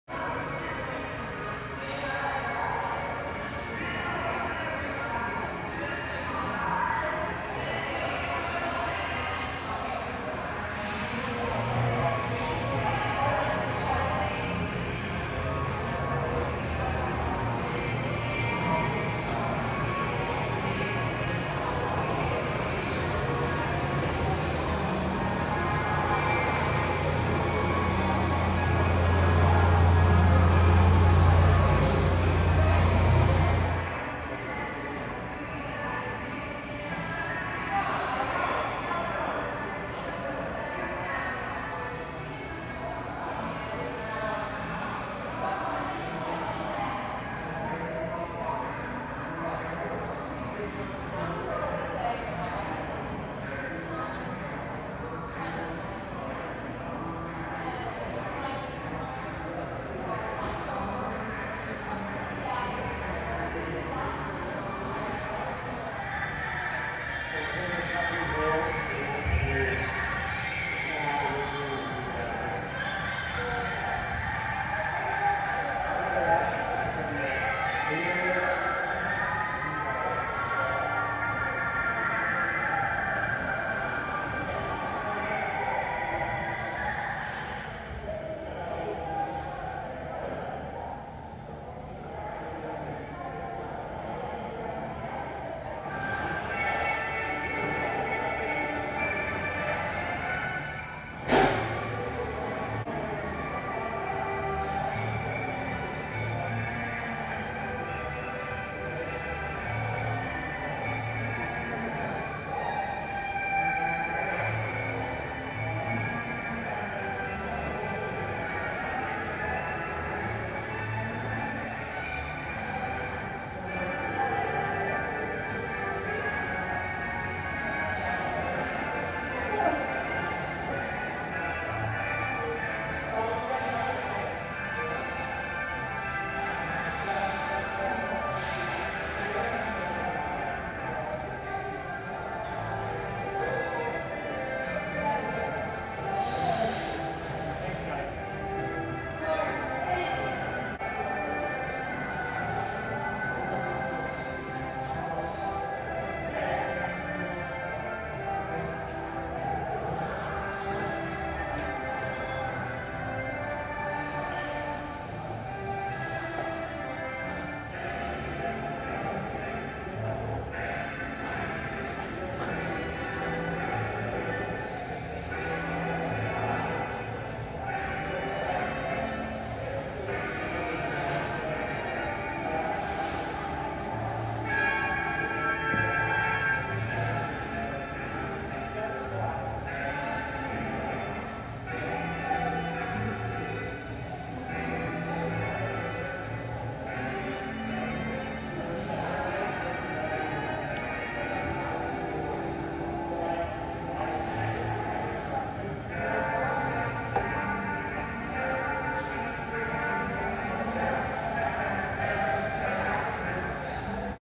Sun valley adult skating week 2020,Lesson three with Brent
Sadly my camera got knocked over right before my lesson started I won’t let that happen again